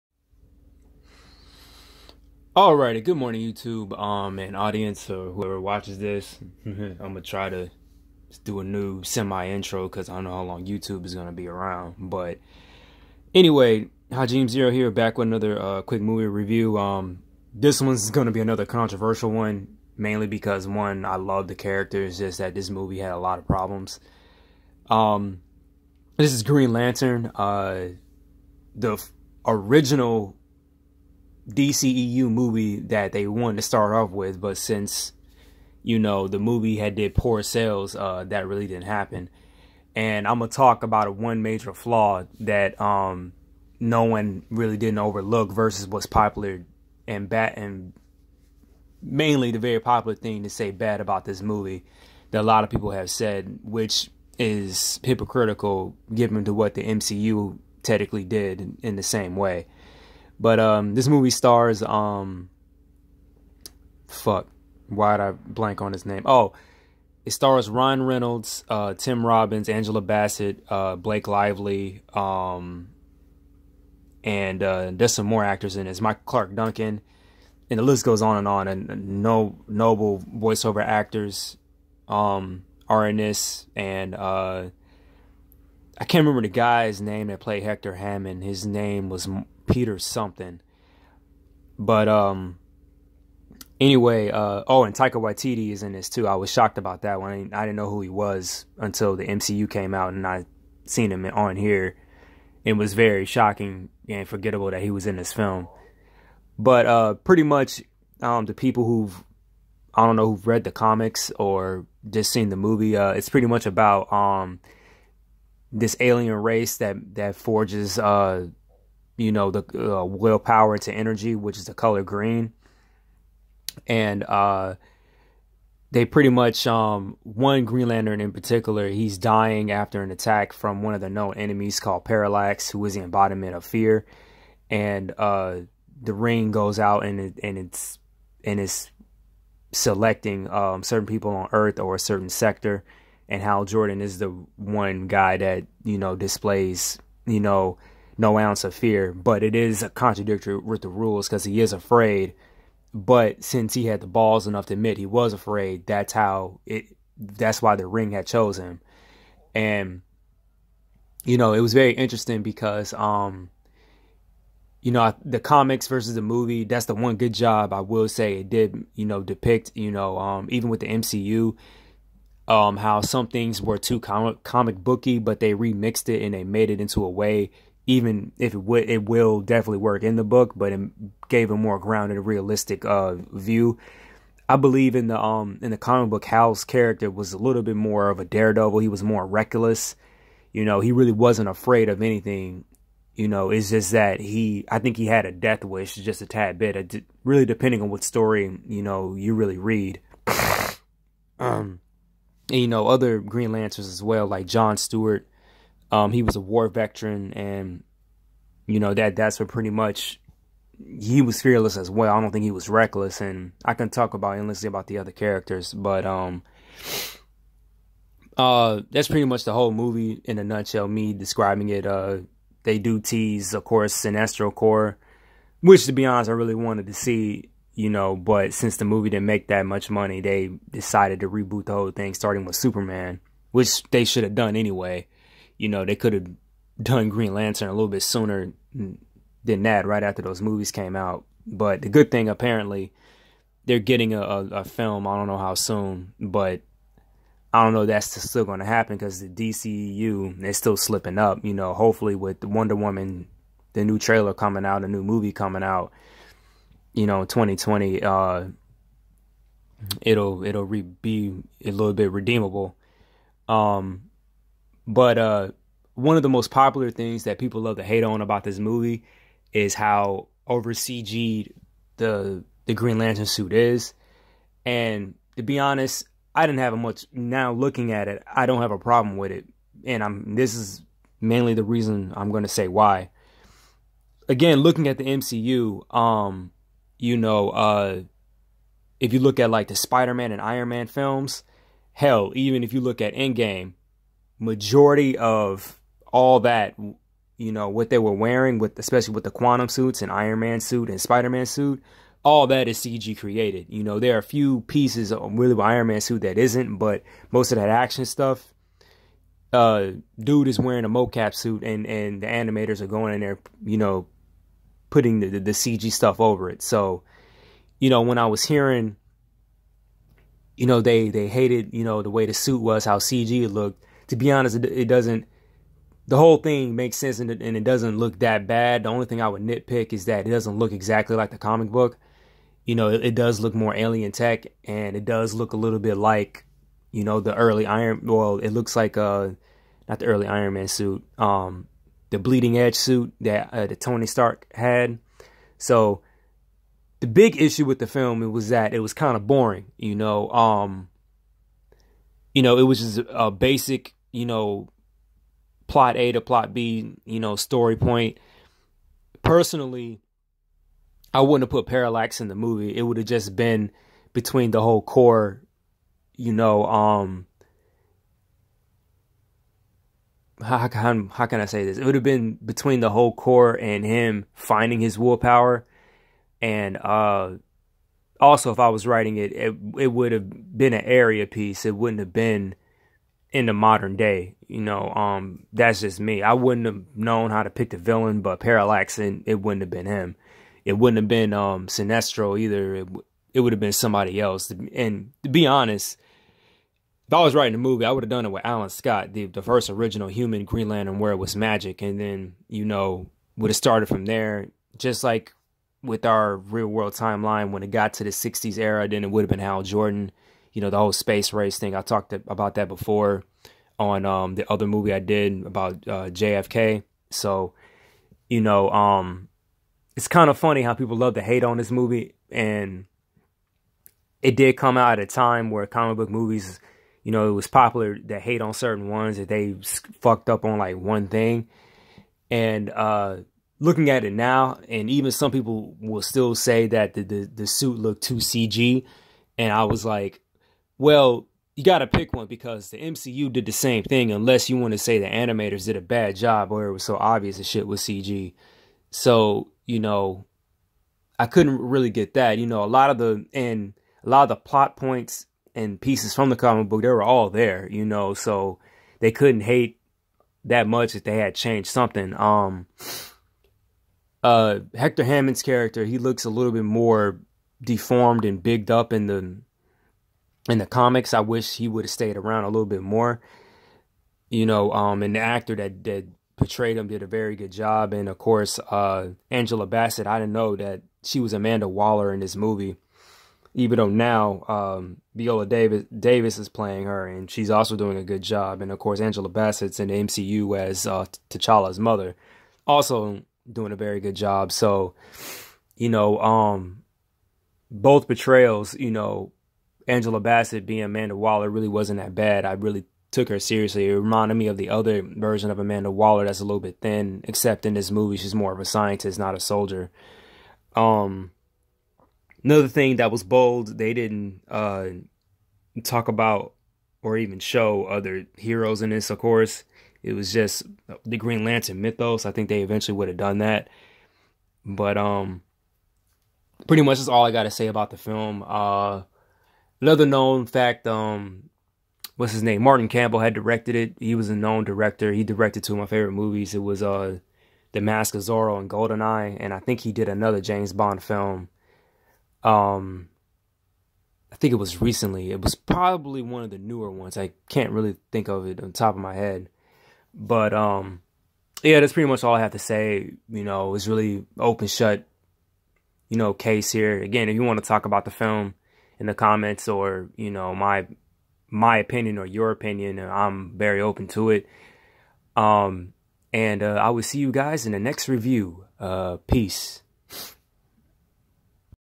GREEN LANTERN QUICK review
SUMMARY: Reckless test pilot Hal Jordan is granted an alien ring that bestows him with otherworldly powers that inducts him into an intergalactic police force, the Green Lantern Corps.

Writers: Greg Berlanti (screenplay), Michael Green (screenplay) Bill Finger(comics), Martin Nodell (comics)

Stars: Ryan Reynolds, Blake Lively, Peter Sarsgaard, Mark Strong, Tim Robbins, Taika Waititi, Angela Bassett, Temuera Morrison, Michael Clarke Duncan, Clancy Brown,